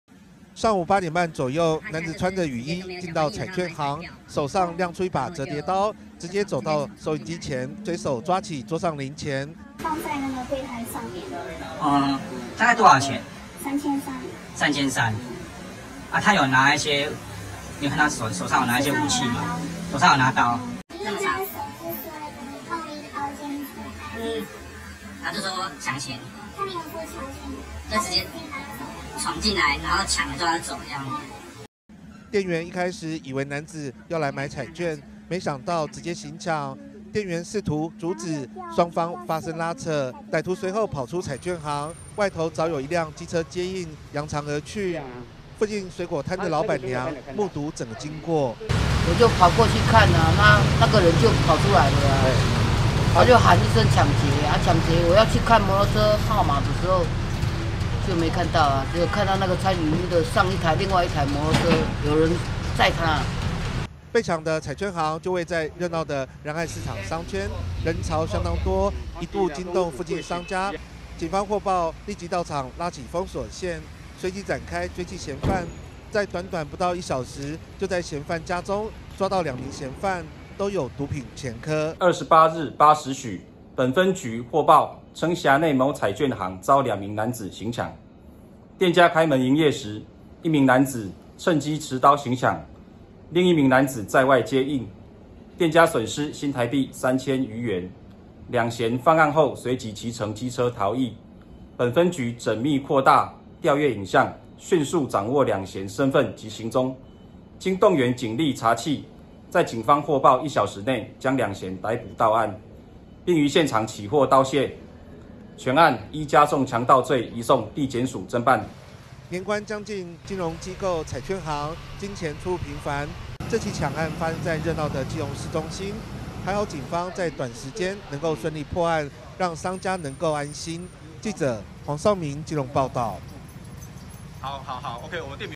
彩券行｜監視器｜搶基隆彩券行 警調監視器1小時逮捕2嫌
(中嘉新聞記者黃紹銘基隆採訪報導) 年關將近，基隆仁四路一間彩券行廿八日發生一起搶案，男子持刀從收銀台搶走3千3百元後，隨後跳上另一輛接應摩托車揚長而去，警方在案發後一小時內，就抓到兩名嫌犯，兩人都有毒品前科，全案依加重強盜罪移送法辦。
早上八點半左右，男子穿著雨衣，進到彩券行，手上亮出一把折疊刀，直接走到收銀機前，二話不說，直接隨手抓起桌上零錢。
店員一開始以為男子要來買彩券，沒想到直接行搶，店員試圖阻止，雙方發生拉扯，隨後跑出彩券行，外頭早就有一輛機車，等待接應，附近水果攤老闆目睹經過。
被搶的彩券行就位於熱鬧仁愛市場商圈，人潮相當多，一度驚動附近商家，警方獲報立即拉起封鎖線，隨即展開追緝嫌犯，短短不到一小時就在嫌犯家中抓到兩名嫌犯，都有毒品前科。
年關將近，金融機構、彩券行，金錢出入頻繁，這起搶案發生在熱鬧基隆中心，一度引起騷動，還好警方順利破案，讓商家能夠安心。